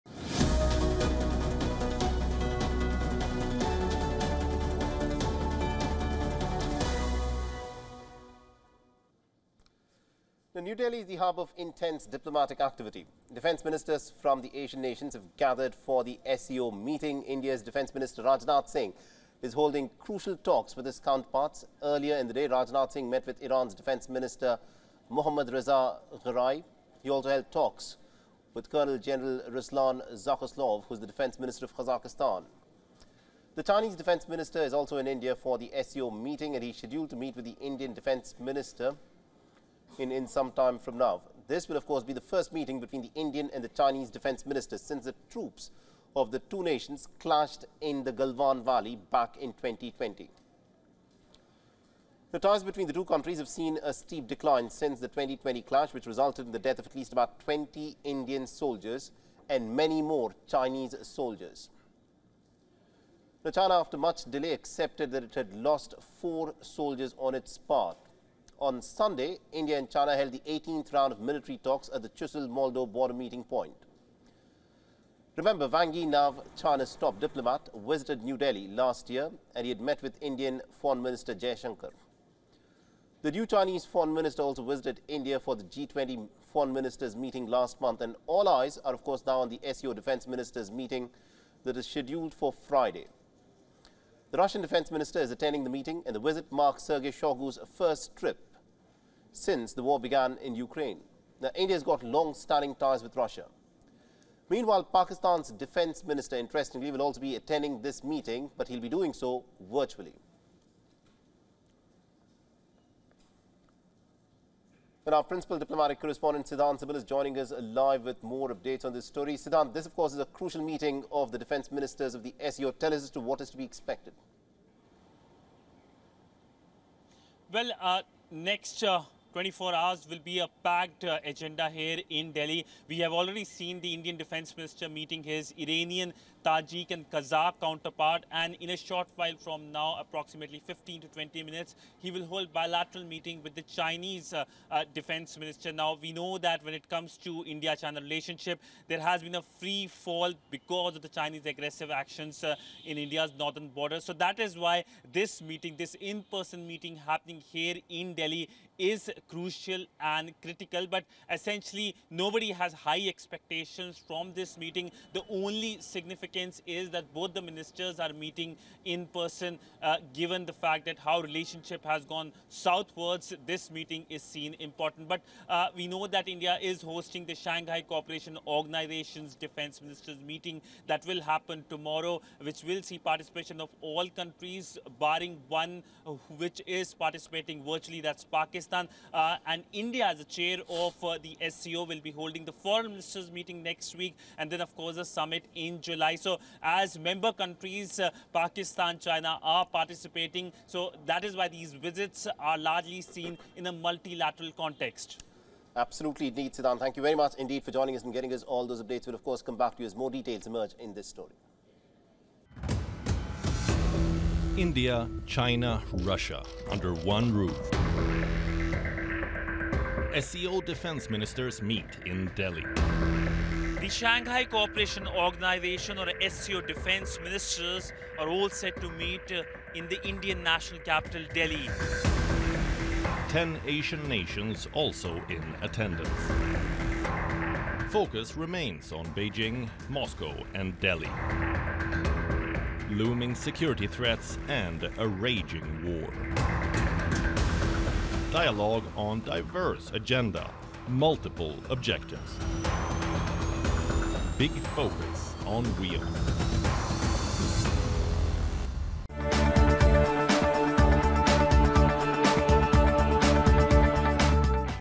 Pak Defence Minister to virtually attend Shanghai group meet in Delhi
Pakistan Defence Minister Khawaja Asif is set to skip his India visit and attend the Shanghai Cooperation Organisation (SCO) Defence Ministers' meeting virtually, sources said.